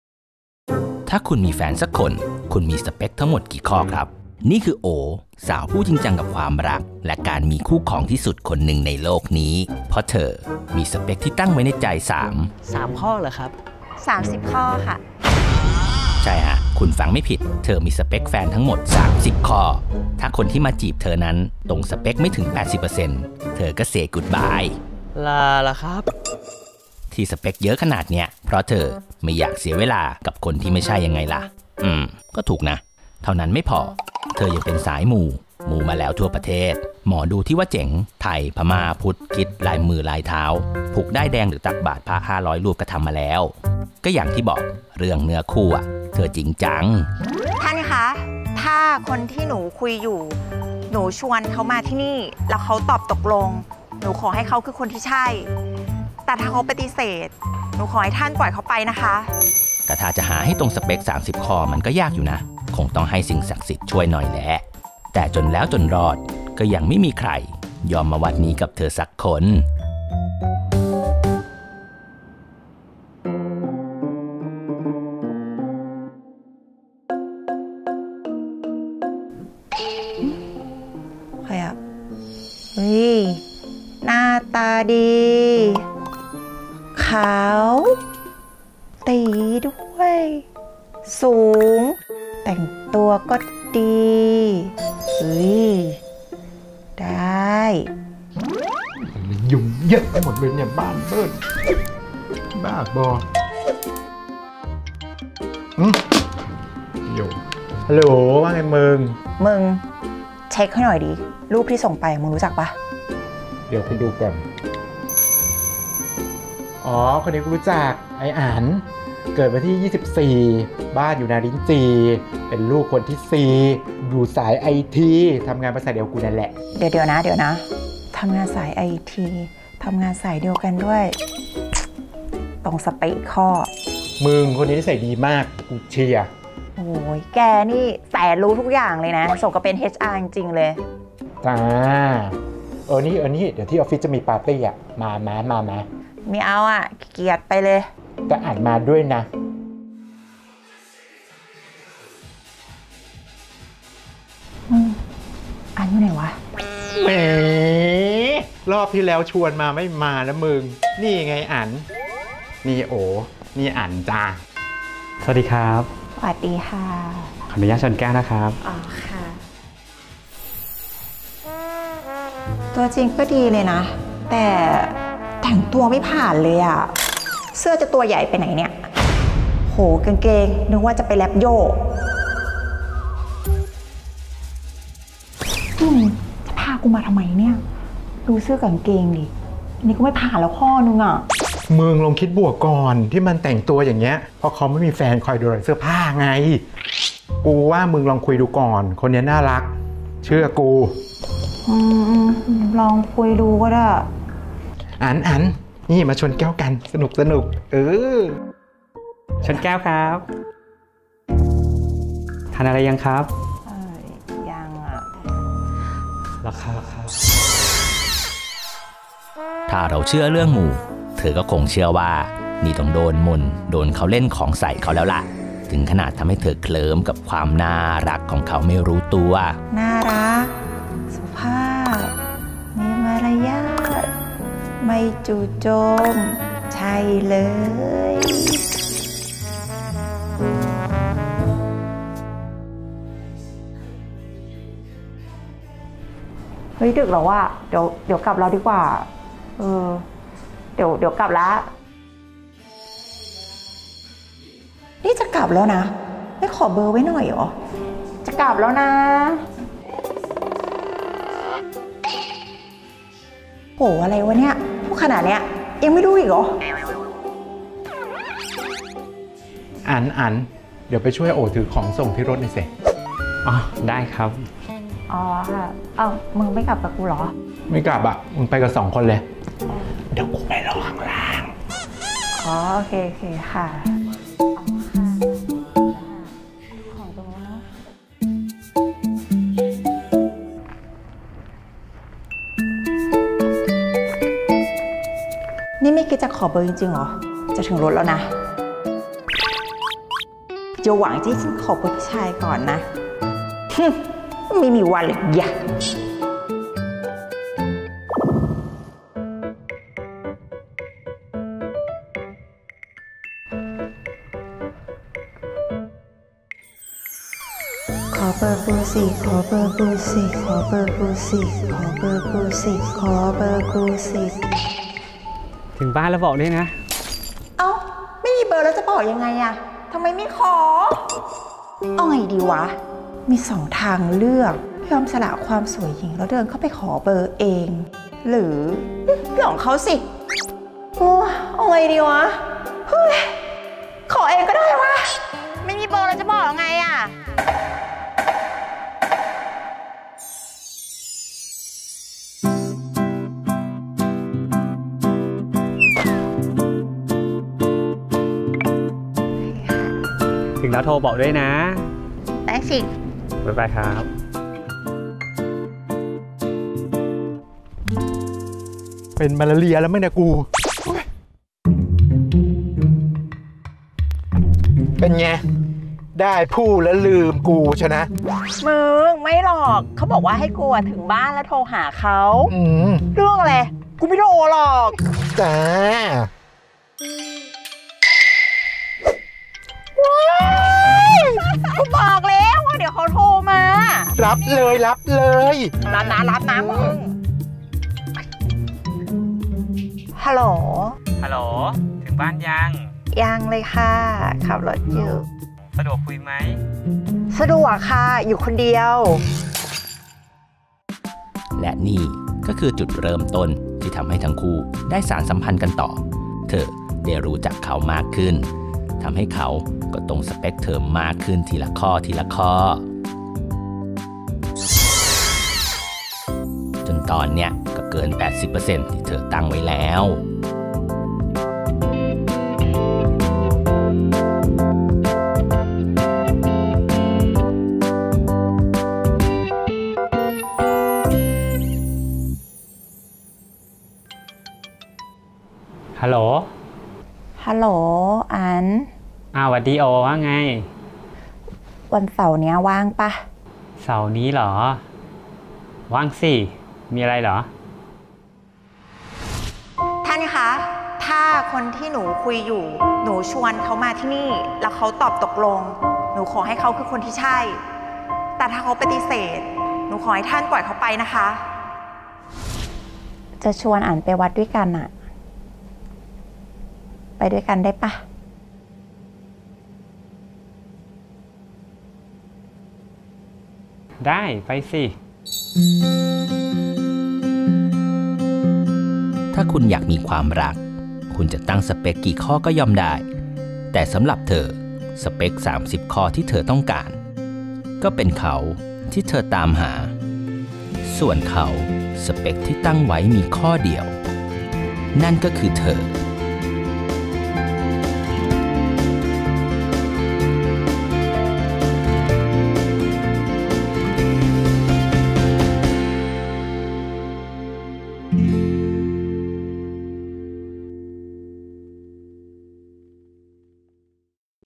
เธอผู้ที่มีสเปค 30 ข้อ กับเขาผู้ที่มีสเปคข้อเดียวนั่นก็คือเธอ.. | โอ๋ x อั๋น Wedding Presentation
เวลาที่คุณตามหาใครสักคน.. คุณตั้งสเปคไว้กี่ข้อ ?
แต่สำหรับเธอ.. เธอตั้งไว้ 30 ข้อ !! 😅
มาดูกันว่าอะไรที่ทำให้เขา.. พิชิตใจเธอได้ 🤩
โอ๋ x อั๋น | Wedding Presentation
เยี่ยมชมผลงานของเราได้ที่
จองเลย !